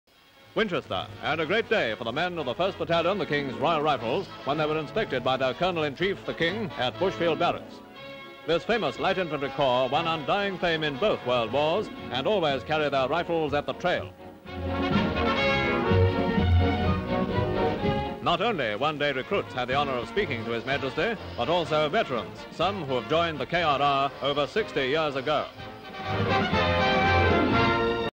ROYAL: The King at Winchester (1950)
King inspects and meets troops at Winchester

Full Description:

ENGLAND: Winchester:
EXT

GEORGE VI KING OF ENGLAND King with mourning band on arm of military uniform for King Gustav pays visit to 1st Battalion King's Royal Rifles at at Bushfield Barracks..Salutes at march past at trot with rifles at trail..Inspects, chats with old veterans and raw recruits in civvies

HARWICK Bramble Island Chemifal Factory blows up with loss of 3 lives...Aerial shots of mus march..and big crater and damage

KING'S ROYAL RIFLES 1st Battalion at Bushfield Barracks Winchester visited by King...inspection,,march past with troops at trot with rifles at trail..chats with veterans and with raw recruits still in civvies

WINCHESTER Visit to Bushfield Barracks of 1st Battalion King's Royal Rifles..inspection, chats to veterans and raw recruits still in civvies..march past at trot with rifles at trail

Royalty,  George VI of Great Britain, army, peacetime,
Background: King inspects and meets troops at Winchester